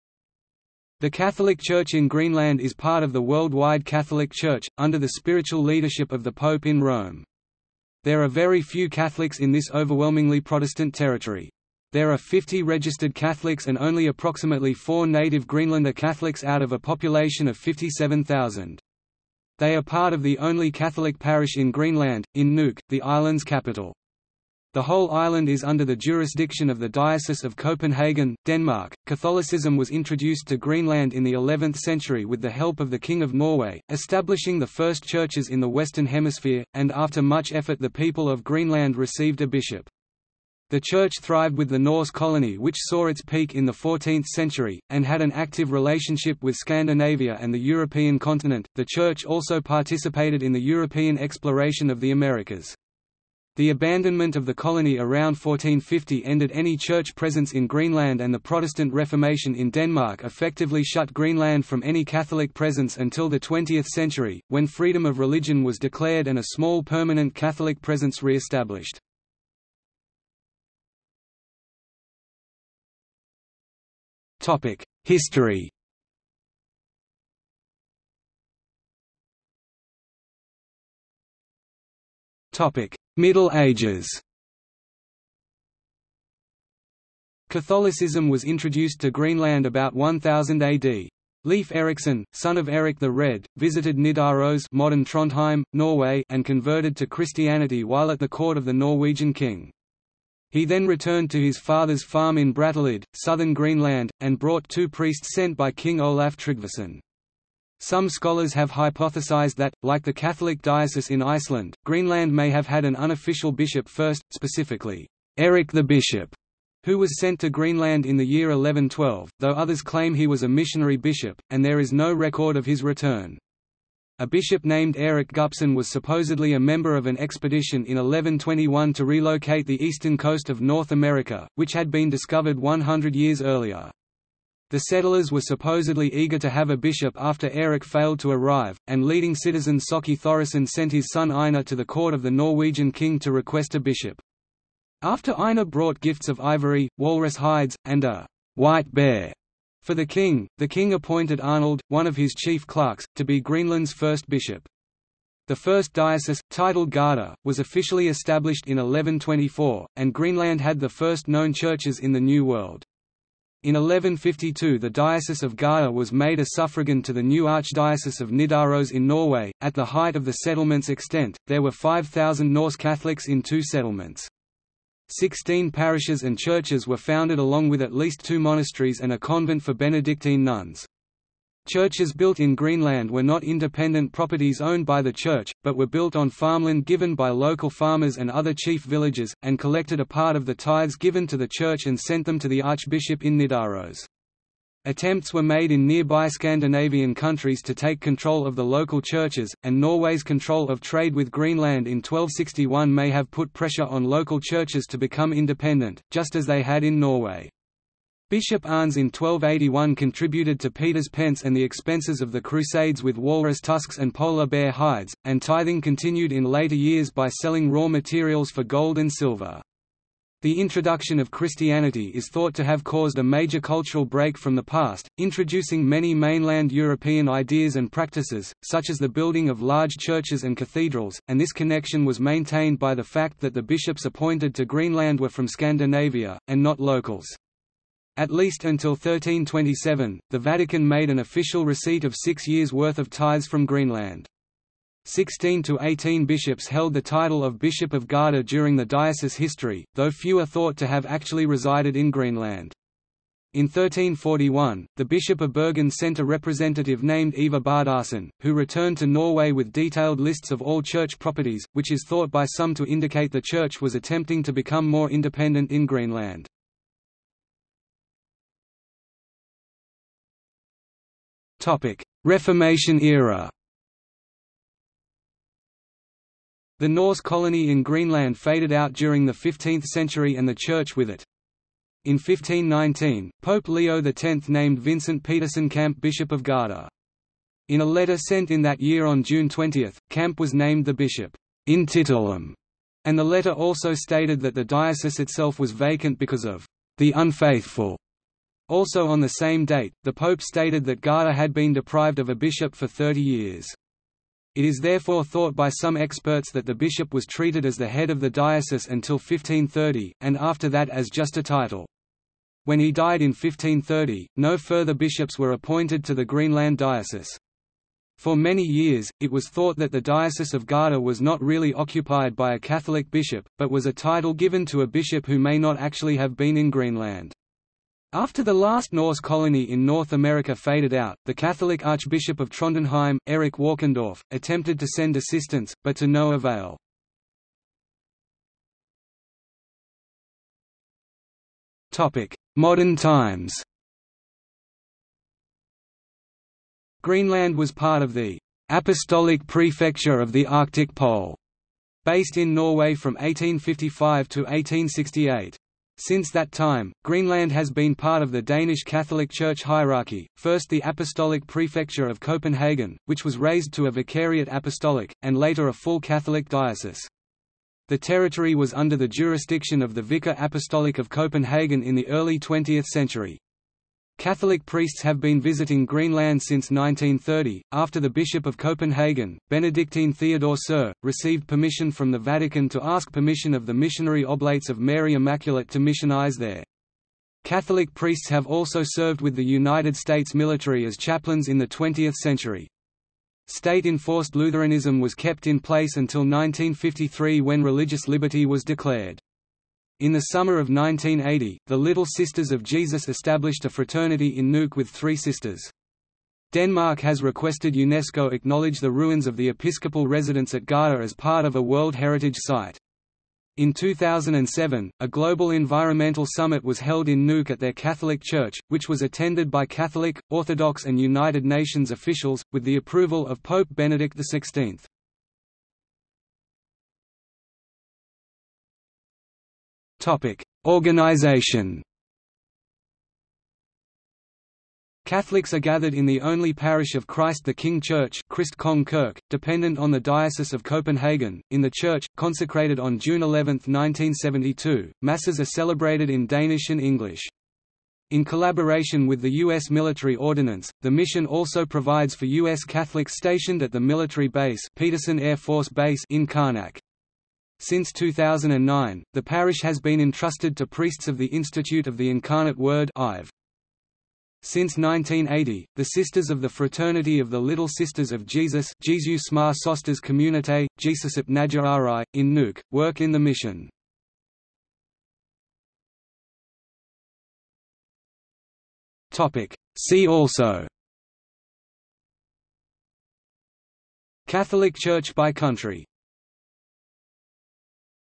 Catholic Church in Greenland | Wikipedia audio article
00:01:10 1 History
00:01:19 1.1 Middle Ages
00:04:28 1.2 Reformation era
00:05:38 1.3 Modern times
00:07:06 1.4 Organization
00:07:55 2 See also

- Socrates

SUMMARY
The Catholic Church in Greenland is part of the worldwide Catholic Church, under the spiritual leadership of the Pope in Rome. There are very few Catholics in this overwhelmingly Protestant territory. There are 50 registered Catholics and only approximately 4 native Greenlander Catholics out of a population of 57,000. They are part of the only Catholic parish in Greenland, in Nuuk, the island's capital. The whole island is under the jurisdiction of the Diocese of Copenhagen, Denmark.Catholicism was introduced to Greenland in the 11th century with the help of the King of Norway, establishing the first churches in the western hemisphere, and after much effort the people of Greenland received a bishop. The church thrived with the Norse colony which saw its peak in the 14th century, and had an active relationship with Scandinavia and the European continent; the church also participated in the European exploration of the Americas. The abandonment of the colony around 1450 ended any church presence in Greenland and the Protestant Reformation in Denmark effectively shut Greenland from any Catholic presence until the 20th century, when freedom of religion was declared and a small permanent Catholic presence reestablished.